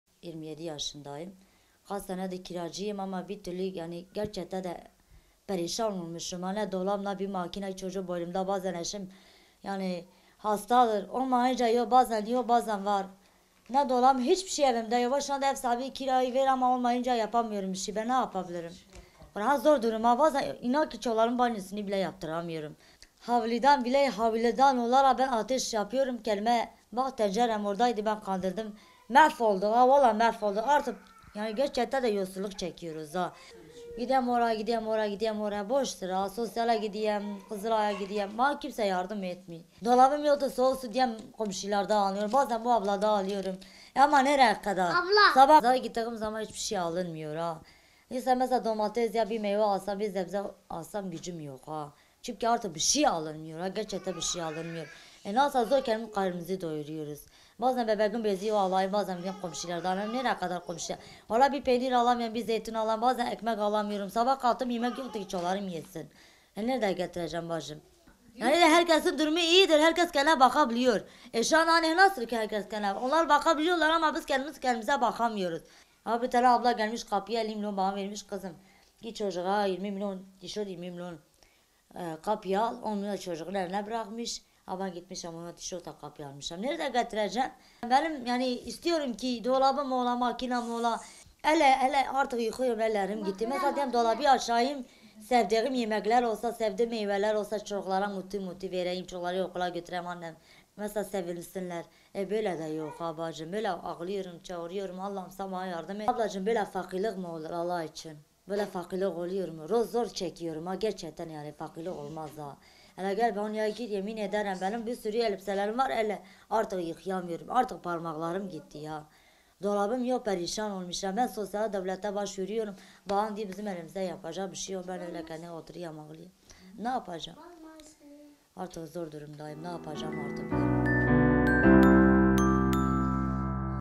ELEKTRİK YOK, YEMEĞİ ATEŞTE PİŞİRİYOR
Kaynartepe Mahallesi’nde yaşayan Sevda Çağlar, üç çocuğuyla elektriksiz evde yaşıyor. Yardımlarla geçinmeye çalışan Çağlar, yemeğini ise avluda yaktığı ateşte pişiriyor.

Diyarbakır’ın Bağlar ilçesine bağlı Kaynartepe Mahallesi’nde yaşayan Sevda Çağlar (27), yokluk içinde yaşıyor. Yıkık, dökük ve elektriği olmayan evde biri 5 aylık, olmak üzere 5 ve 6 yaşında 3 çocuğuyla yaşayan Sevda Çağlar, eşinden boşandığı için çocuklarını yalnız büyütüyor.

YARDIMLARLA GEÇİNİYOR

İşsiz olduğunu söyleyen Çağlar, “Yıllardır kiracıyım, perişan olmuşum. Evimde ne dolap ne makine var, hiçbir şey yok. Alacak gücüm de yok. Çok zor durumdayız, bazen çocuklarımın banyosunu bile yaptıramıyorum. Tüpüm dahi yok. Evimin avlusunda ateş yakıp, yemek yapıyorum. O da malzeme olursa. Çünkü hiçbir şey alınmıyorum. Yardım için her yere gidiyorum ama eli boş dönüyorum” dedi.